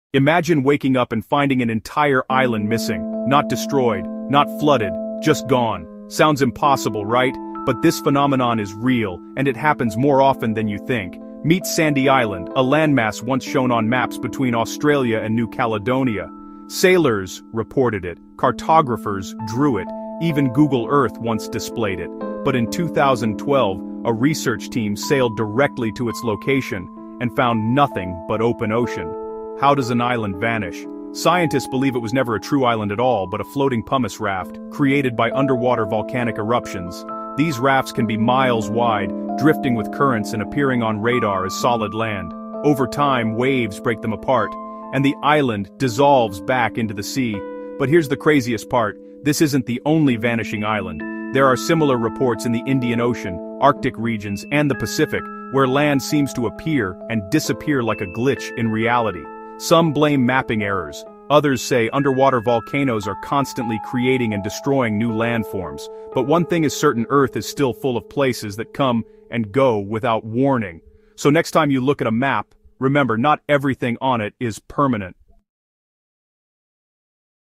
The Mysterious Island That Vanished Overnight No One Expected This…
Uncover the eerie mystery of Sandy Island a place sailors once saw, maps recorded, and satellites confirmed… yet it doesn’t exist. Scientists discovered that Earth still hides places that appear and disappear like glitches in reality. From volcanic pumice rafts to oceanic illusions, this story reveals how unstable our planet truly is.
Dive deep into the science and secrets behind Earth’s vanishing lands.